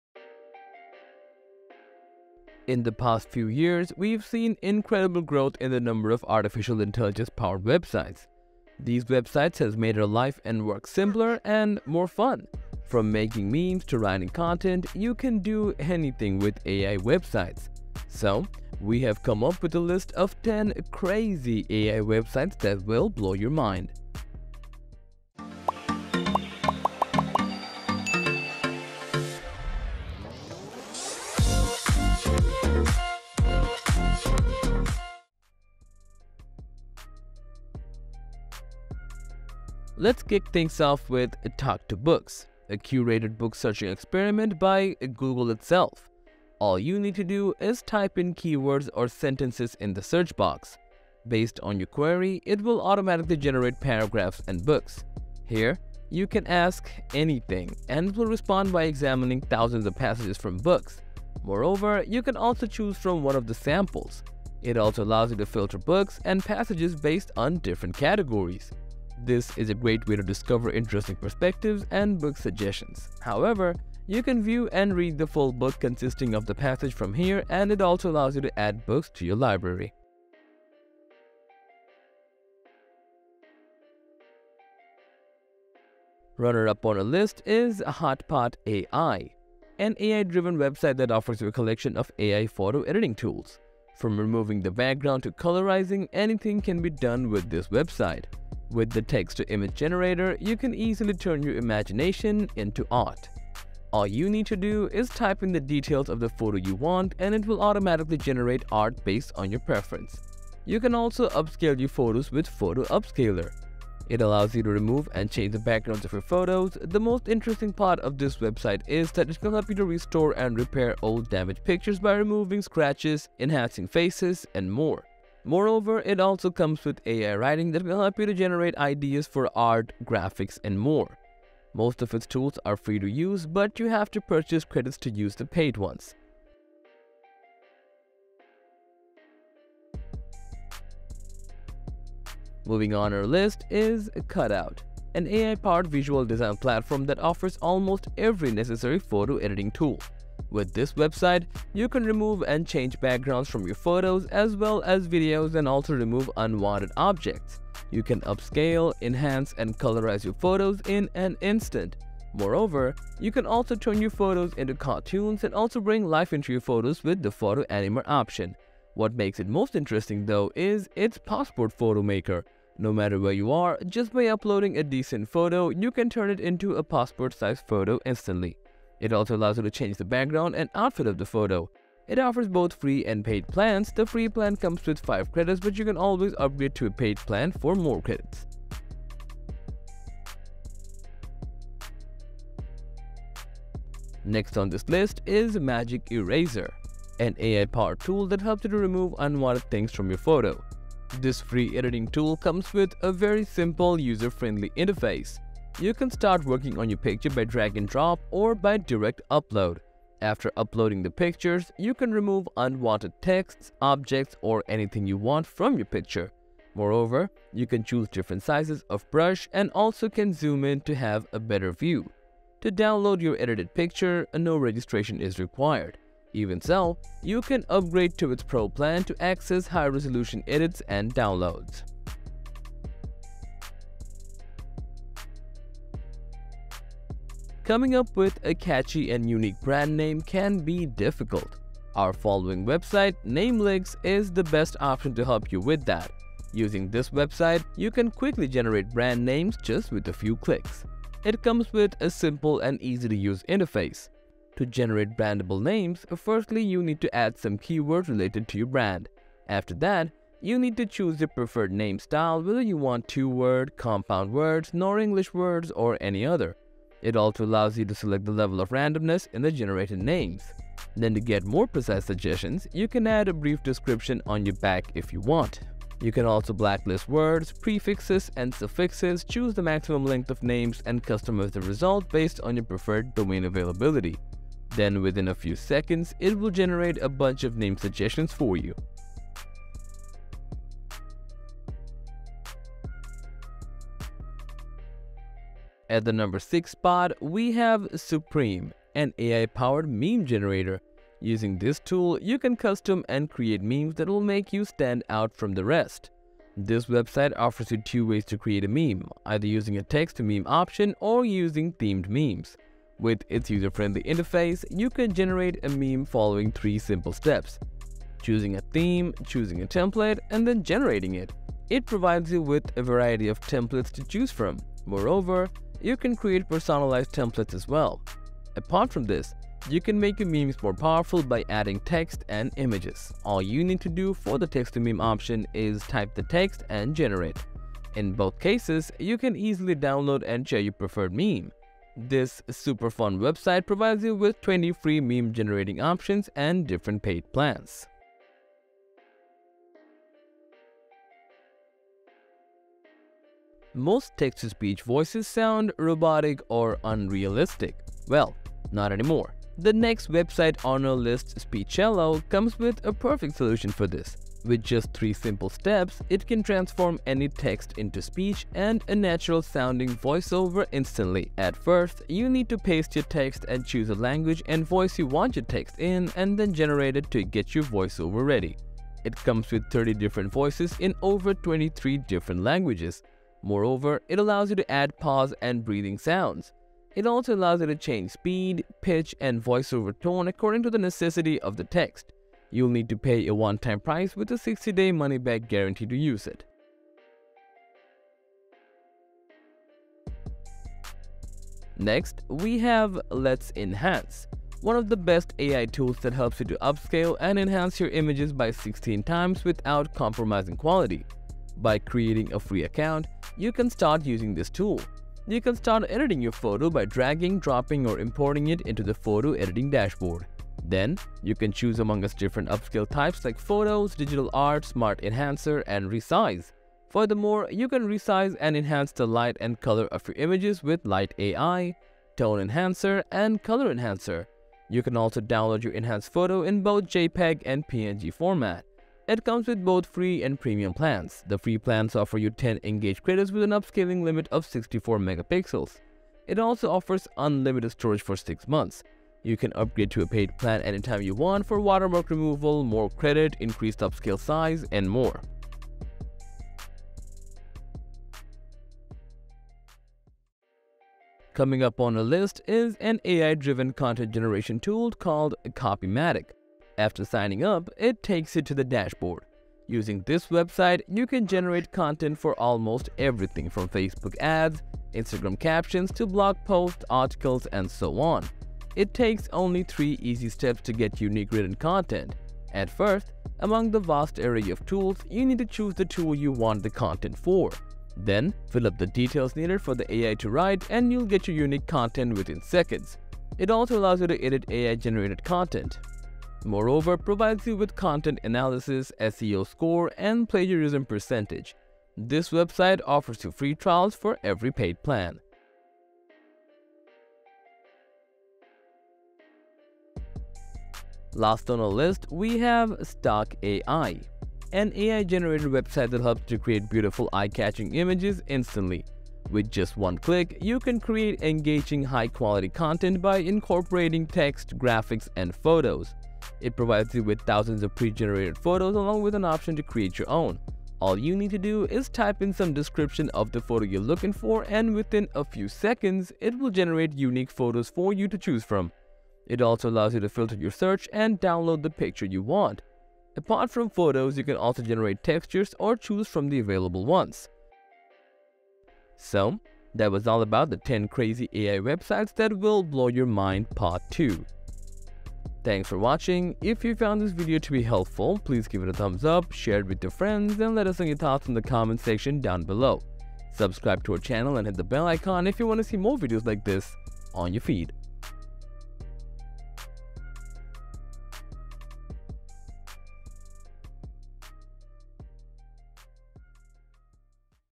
10 Crazy AI Websites That Will Blow Your Mind ▶2
Here are 10 crazy AI websites that will help you generate a deep fake voice, create unique color palettes and convert text into animation in just a matter of seconds. But there are more. So watch this video till the end where we will show you 10 crazy AI websites that will blow your mind.

Crazy AI Websites That Will Blow Your Mind ▶2

Talk to books

Hotpot

Cutout

Magic eraser

Namelix

Super meme

Speechelo

Let's enhance

Copymatic